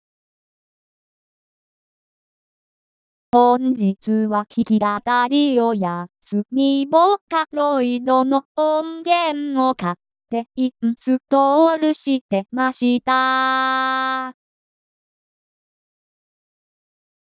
実験また実験（VOCALOID3 IA ROCKS）
まあ色々やってますがｗｗｗ
ボカロPは目指してないッスｗｗ
それは間違いない！
てか、使いこなせる気がしませんもん！
根気のあるひとでないとダメな気がします。

あ、音源はこれしか無かったので。
某S村楽器ですが、大半は取り寄せなんだって。
唯一これだけが空箱じゃなかったっていう。

とりあえず歌ってくれるトコロ確認で
音階はめちゃくちゃに入れて、歌詞挿入。